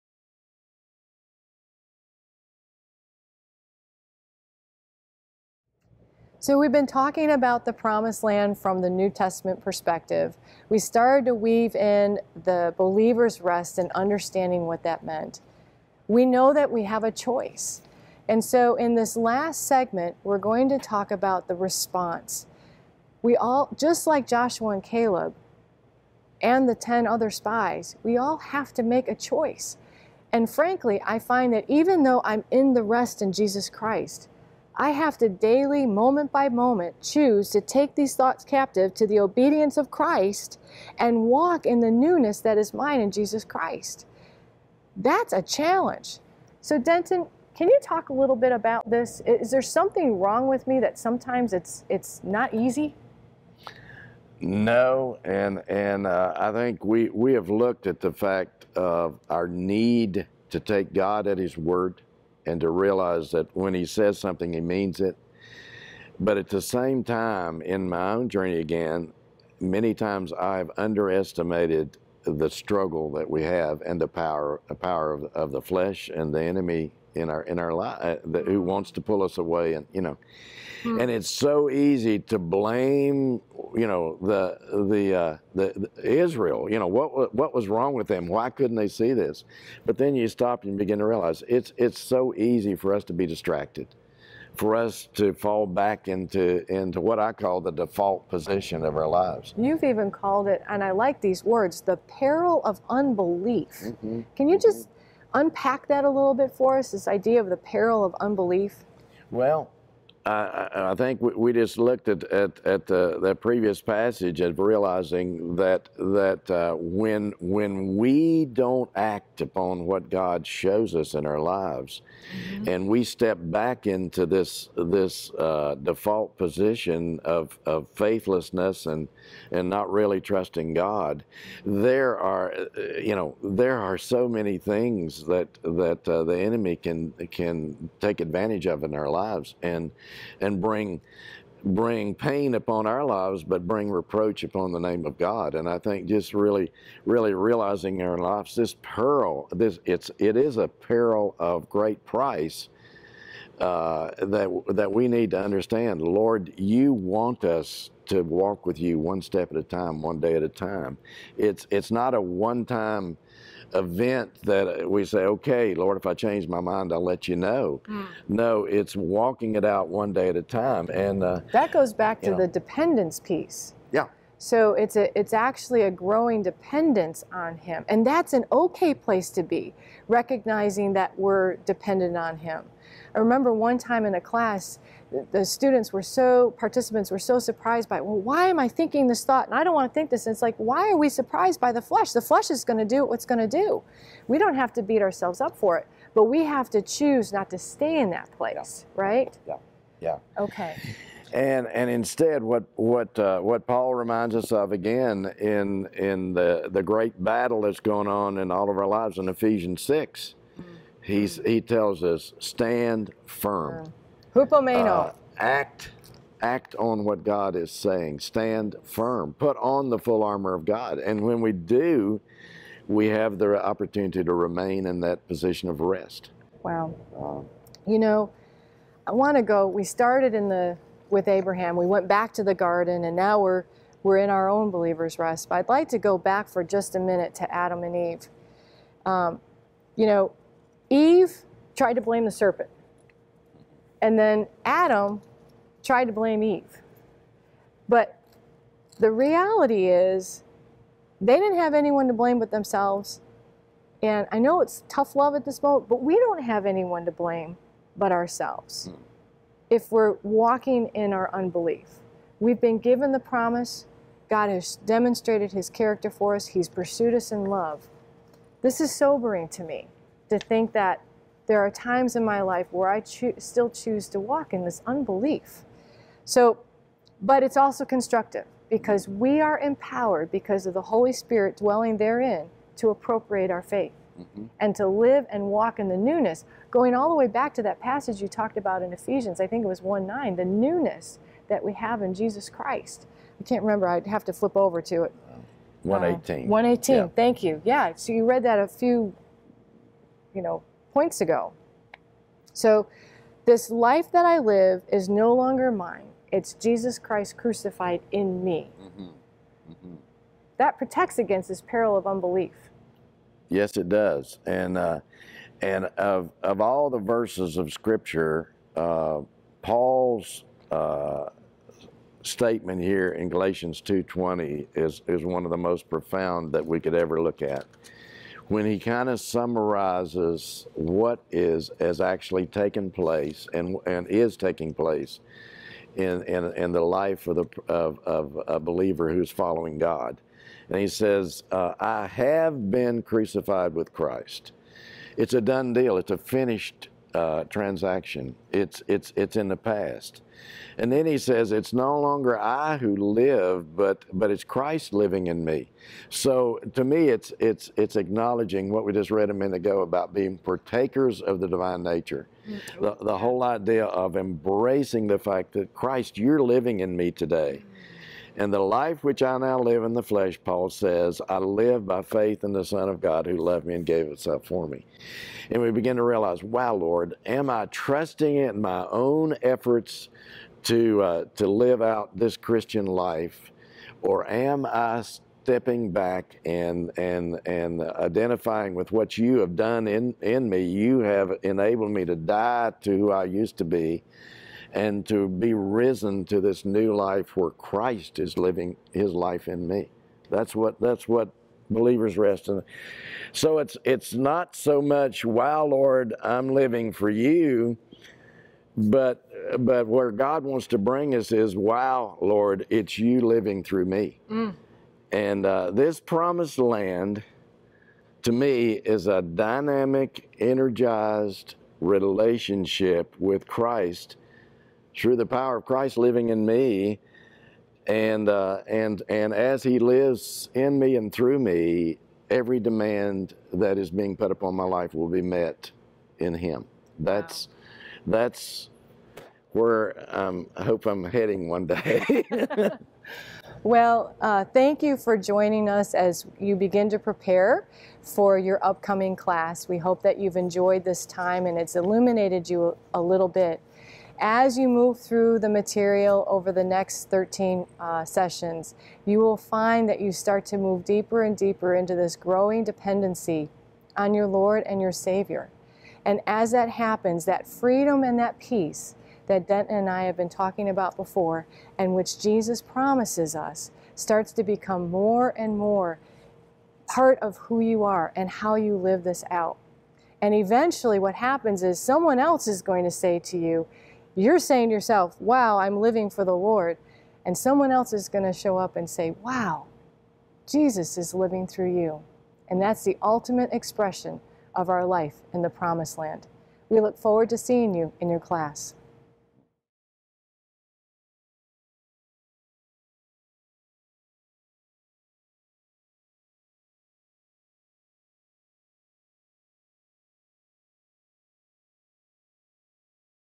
PLL Intro Part 2.5 Daily Response of Walking with Christ
It's so easy to get distracted and to fall back into the default position – what was life like before Christ.  Just like the Israelites desire to fall back to a desire to go to Egypt.  The peril of unbelief is real.  It leads to a perpetual wandering, lack of peace and bondage.   We say we don’t want to live that life, but the question remains as the Hebrew author writes -.  Do I really believe?  Our relationship with Jesus is a one-time event (salvation) with a lifetime response (sanctification).  Hebrews  3:14 -  When we abide, even when forget or doubt or question, we are living by faith.  There is a solemn reminder to remain in the rest.